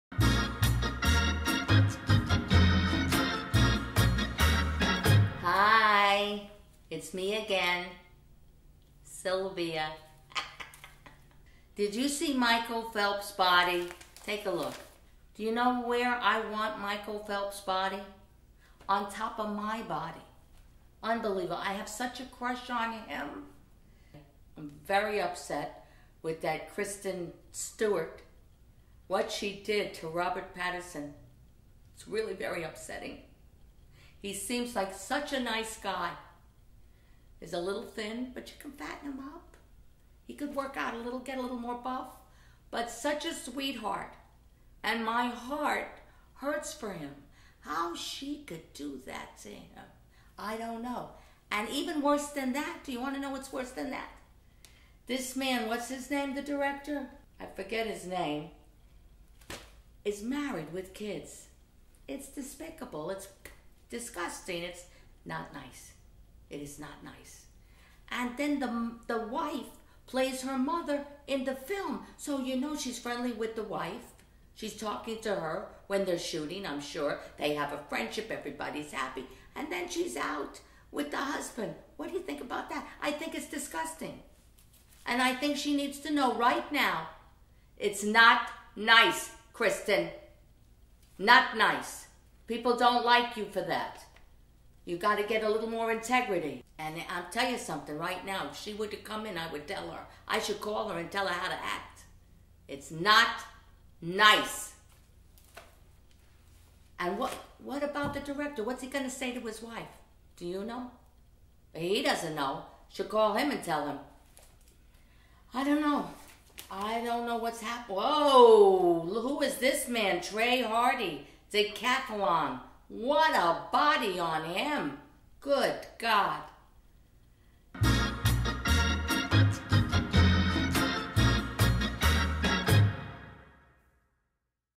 Sylvia's Just Sayin'...Michael Phelps & Kristen Stewart [& Trey Hardee]
Laying out the hard truths on Michael Phelps and Kristen Stewart...It's not nice, Kristen! Oh, and there goes Mistah Trey Hardee to save the day...decathalon, huh?